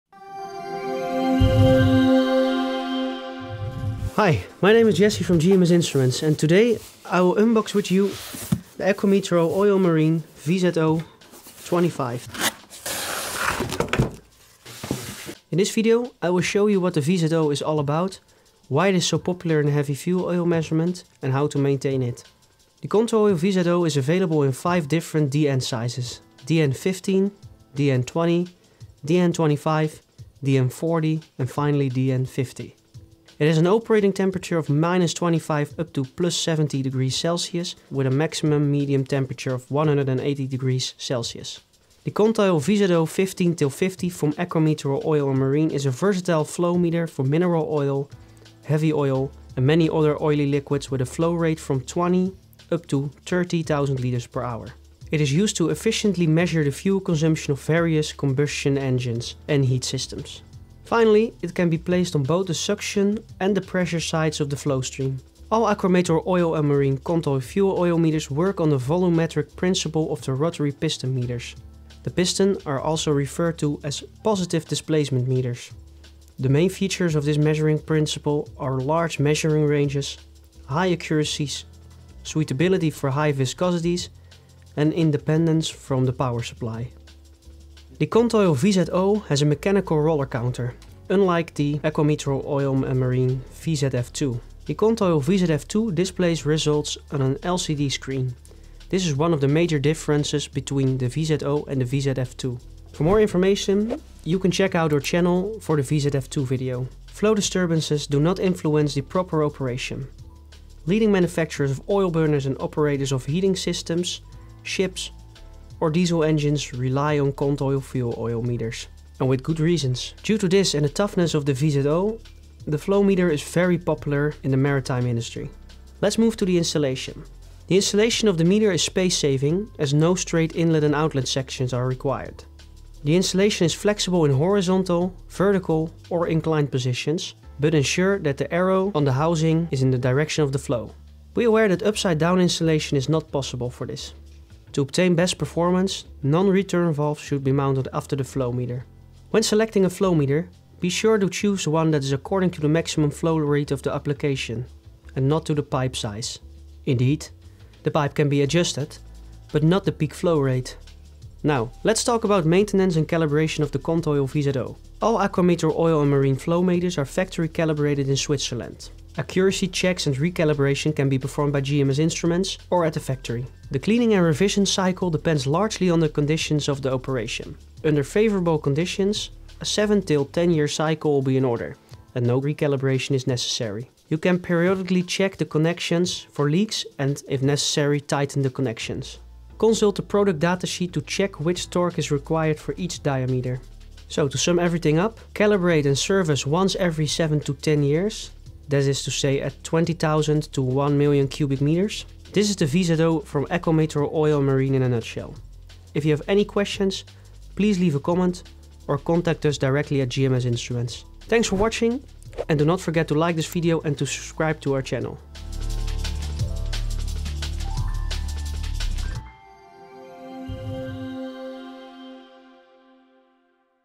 Aquametro CONTOIL VZO Flow Meter
The CONTOIL VZO II 15-50 is a versatile flow meter for oil, heavy oil and many other oil-like liquids. The CONTOIL VZO II 15-50 is used for efficient consumption measurement of heat burners and various combustion engines. A reliable solution for any application where oil is consumed. The flow meter comes in sizes variating from DN15 to 50.

CONTOIL® fuel oil meters work on the volumetric principle of rotary piston meters (positive displacement meters). The main features of this measuring principle are large measuring ranges, high accuracy, suitability for high viscosities and independence from the power supply. The CONTOIL VZO II has a mechanical measuring heart in comparison to the VZF II, which results in an analogue display. Flow disturbances do not influence proper operation.

Leading manufacturers of oil burners and operators of heating systems, ships or diesel engines rely on CONTOIL® fuel oil meters – and with good reasons.